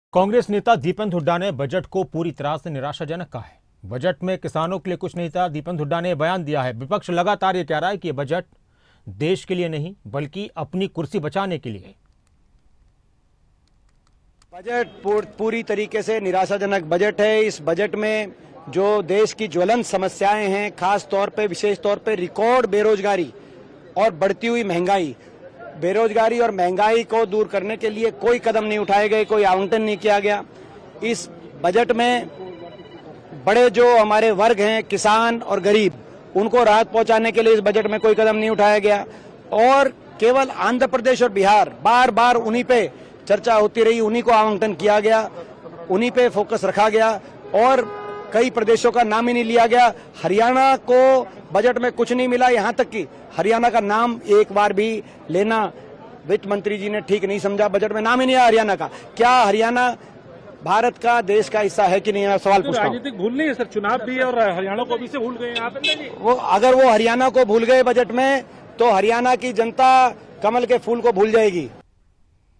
दीपेंद्र हुड्डा ने बजट को बताया निराशाजनक, बोले- 'बजट में हरियाणा का जिक्र ही नहीं हुआ'
'आंध्र प्रदेश और बिहार पर हुई बार-बार चर्चा'